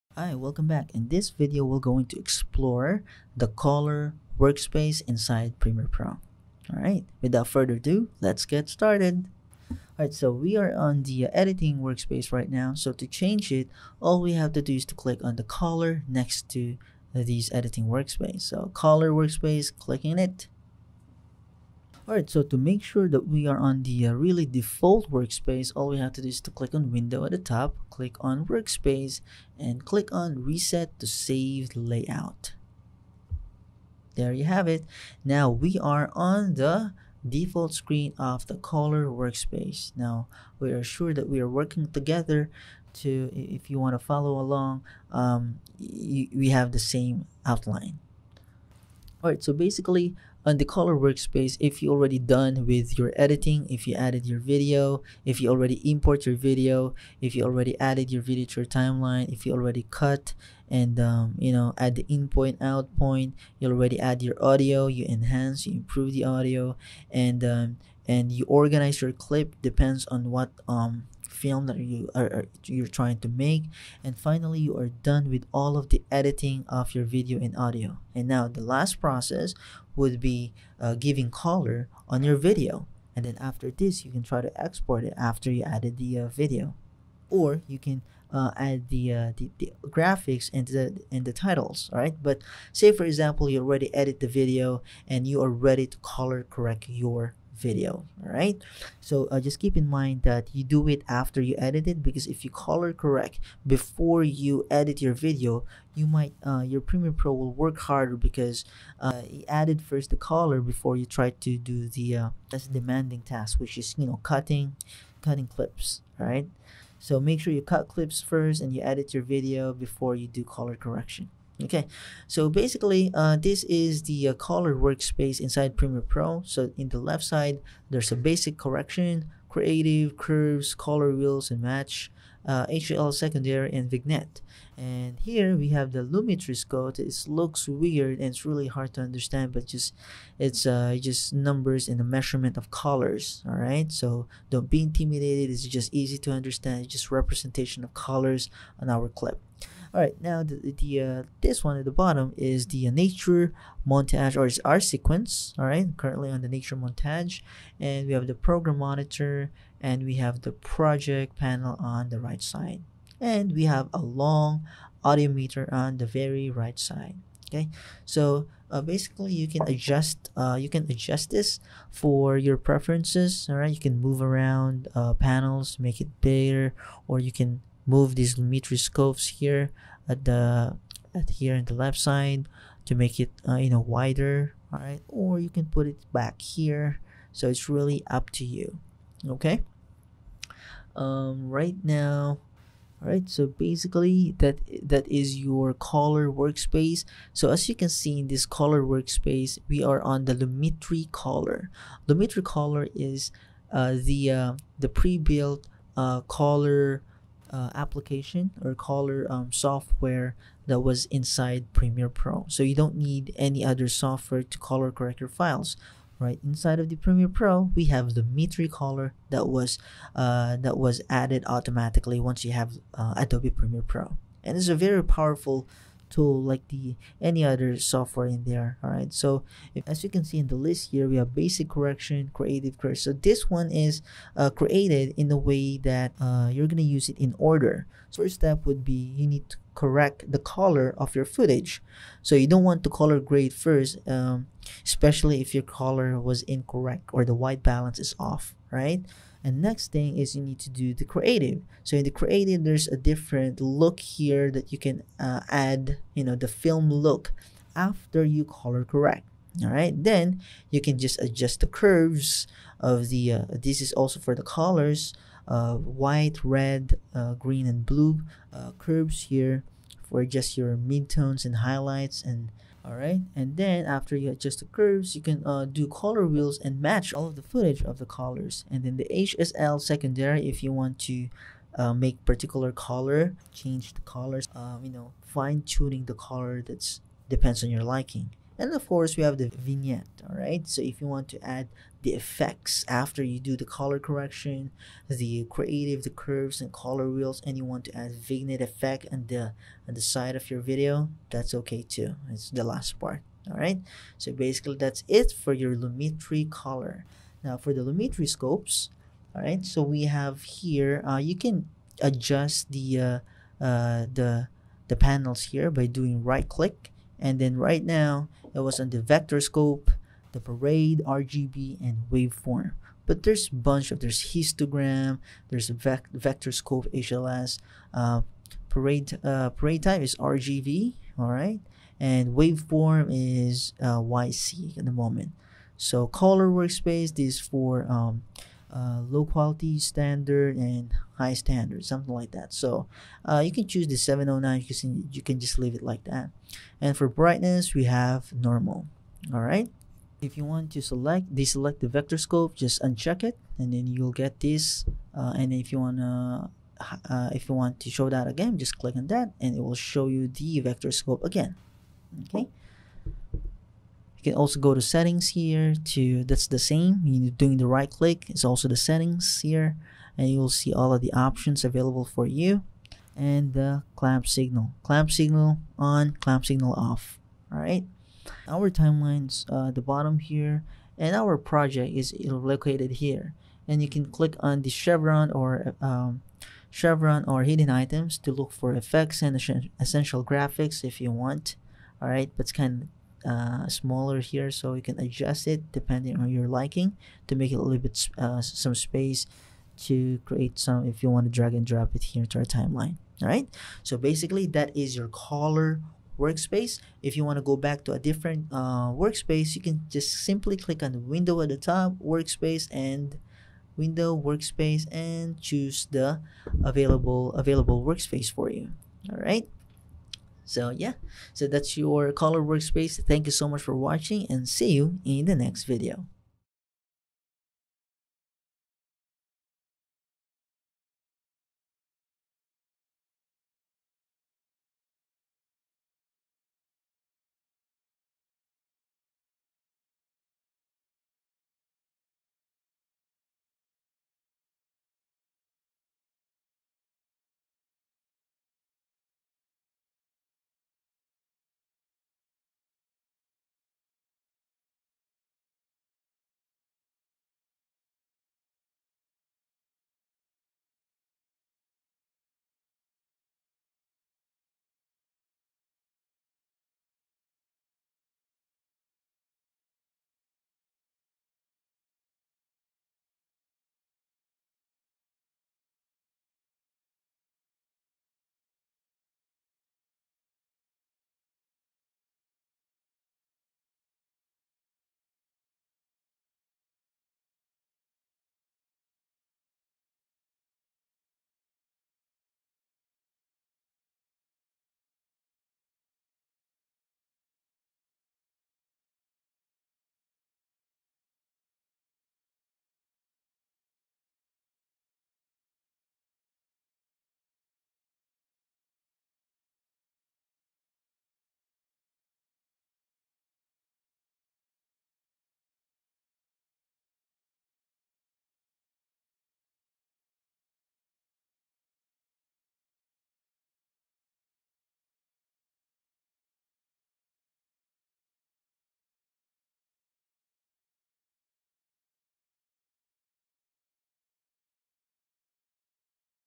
How to Use Color Workspace in Premiere Pro CC 2020
To use the Color Workspace in Premiere Pro CC 2020, follow these steps:

1. Open your project in Premiere Pro CC 2020.
2. Ensure that you are in the Editing workspace.
3. Go to the top menu and click on "Window."
4. From the drop-down menu, select "Workspaces" and then choose "Color."
5. The interface will switch to the Color Workspace, which is specifically designed for color grading and correction.
6. In the Color Workspace, you will see the Color Panel, which provides various tools and controls for adjusting and enhancing the color of your footage.
7. Explore the different sections of the Color Panel, such as the Lumetri Color, Color Wheels, Color Curves, and Color Adjustment tabs.
8. Use the Lumetri Color panel to make global adjustments to the overall color, exposure, contrast, and saturation of your footage.
9. Use the Color Wheels and Color Curves to fine-tune the shadows, midtones, and highlights, and make targeted adjustments to specific color ranges.
10. Experiment with the various controls and settings in the Color Panel to achieve the desired look for your footage.
11. Additionally, you can save and apply color presets and effects to streamline your color grading workflow.

By using the Color Workspace in Premiere Pro CC, you can have precise control over the color and achieve professional-looking results in your videos.

For a more in-depth understanding of Premiere Pro CC and to explore advanced techniques, enroll in our comprehensive Udemy online course

Take your editing skills to the next level and become a Premiere Pro expert today!